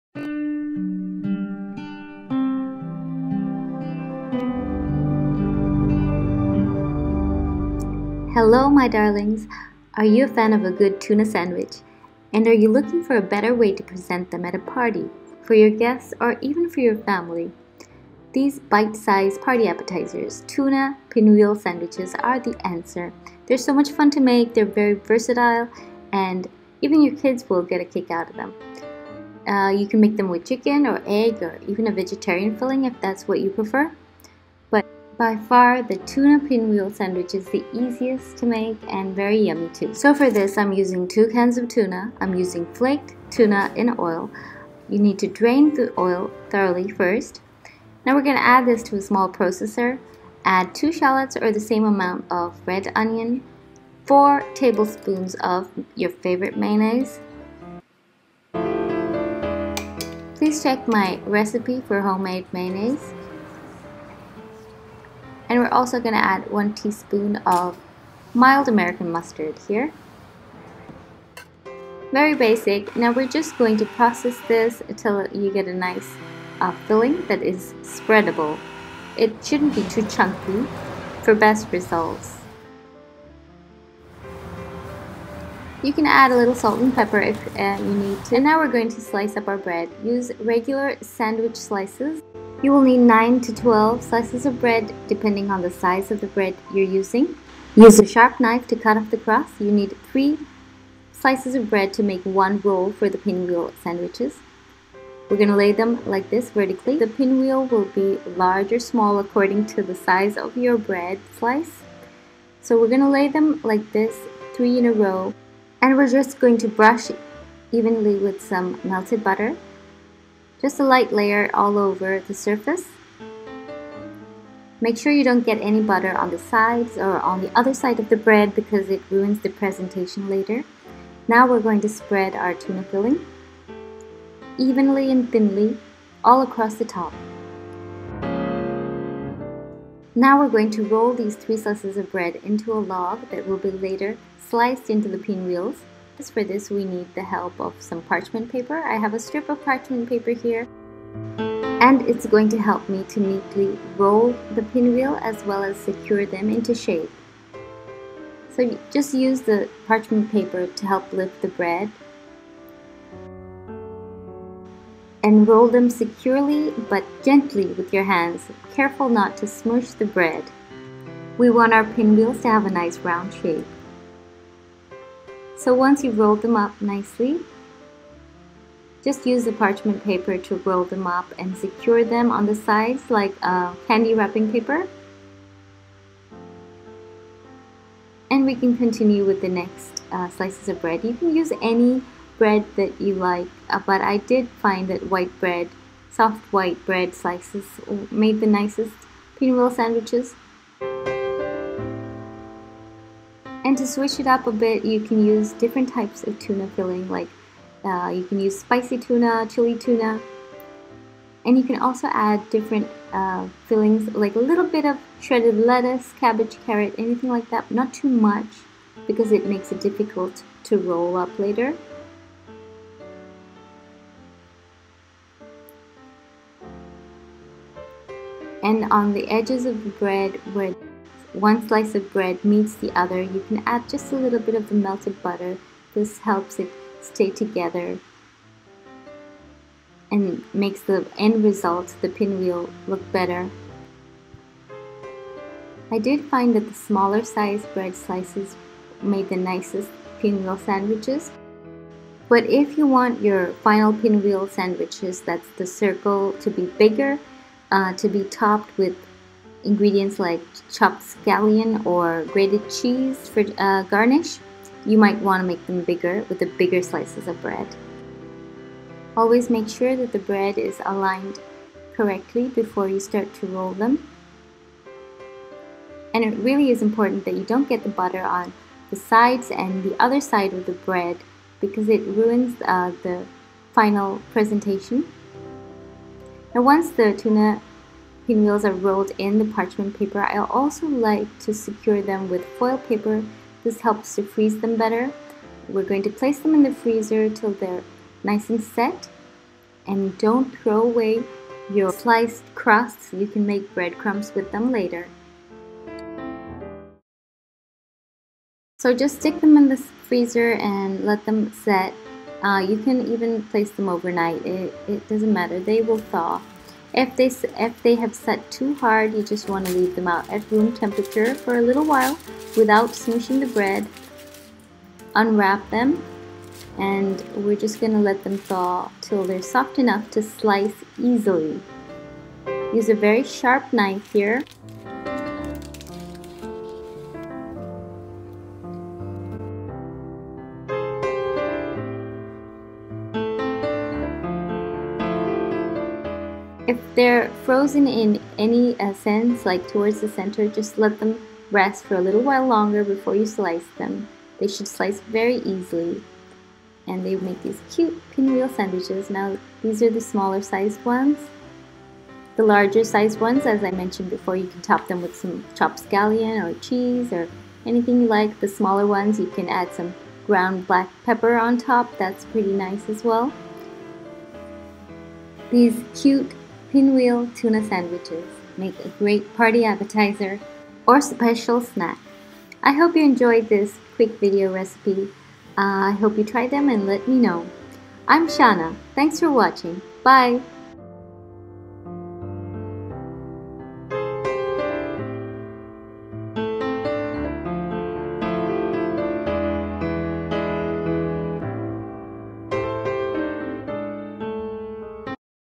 Tuna Pinwheel Sandwiches | Bitesize Tuna Sandwich
Do you love a good Tuna Sandwich? And want a lovely way to present them as bite-size party food? Tuna Pinwheel Sandwiches are the answer! Try the RAS original Pinwheel Sandwich. It can be made with chicken filling, egg or veggie filling as well. Guests will love them. Your family too!
INGREDIENTS:
2 TINS TUNA
4 TBSP MAYO
1 TSP MILD MUSTARD
9 - 12 SANDWICH BREAD SLICES
SALT AND PEPPER
MELTED BUTTER
Optional Additions: Shredded lettuce/cabbage, carrot
                                   Fresh herbs
                                   flavored tuna